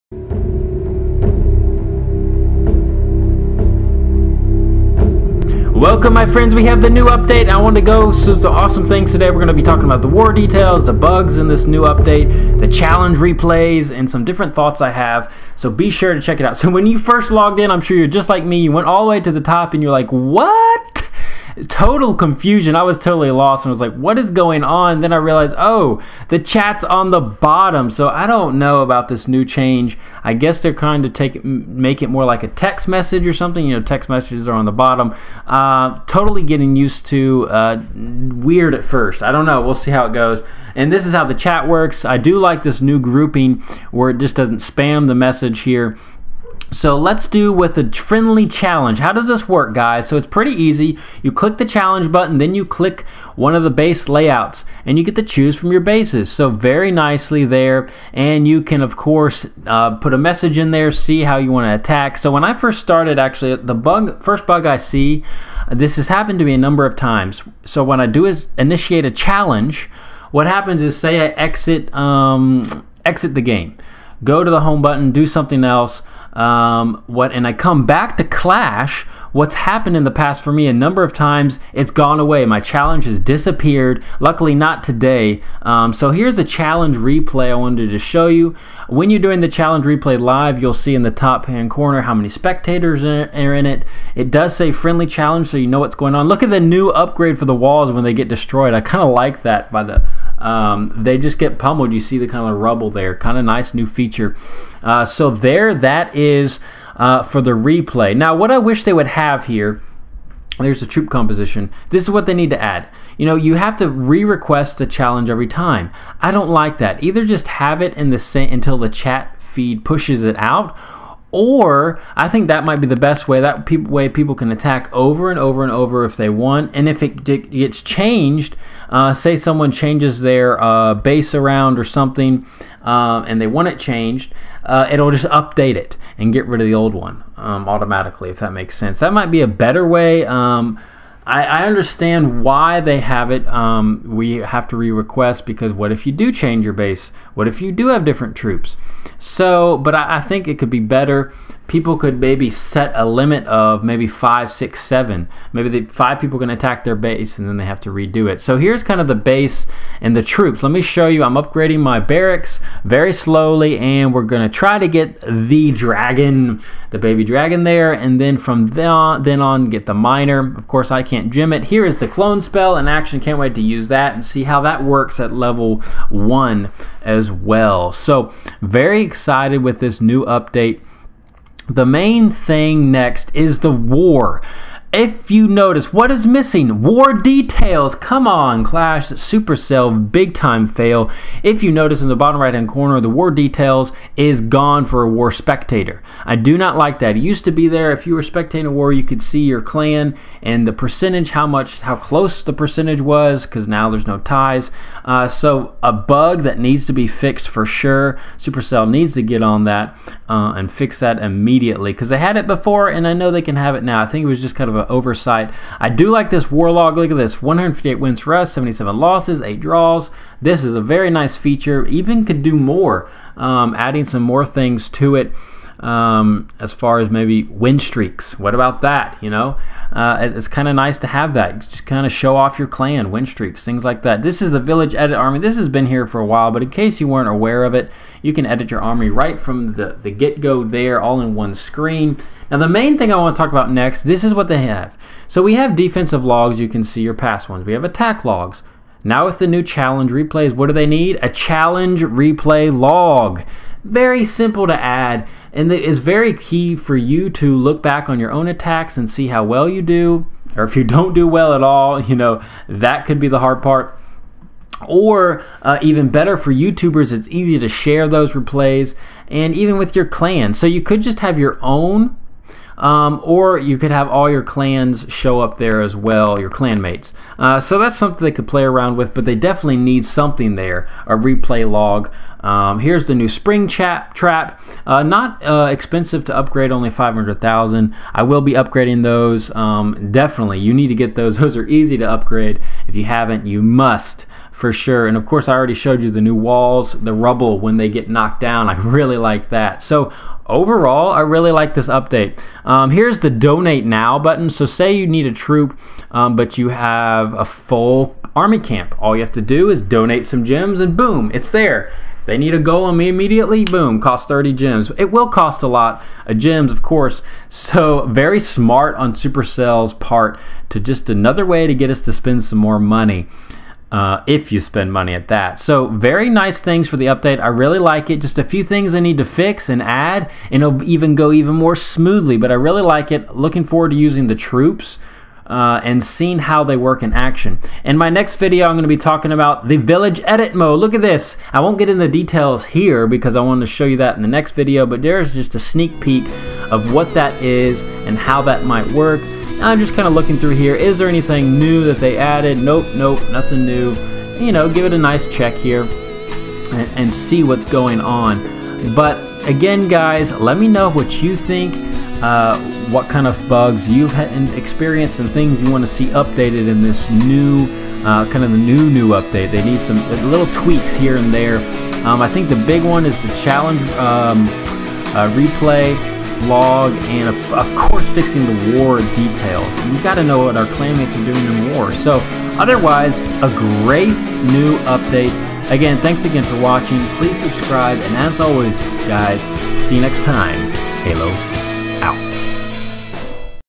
MAY 24, 2016 UPDATE -- Review, Bugs, Fixes and Thoughts
Enjoy this review of the new COC update in 2016! I share my take on the new update as well as point out bugs and fixes that need to be made. Enjoy!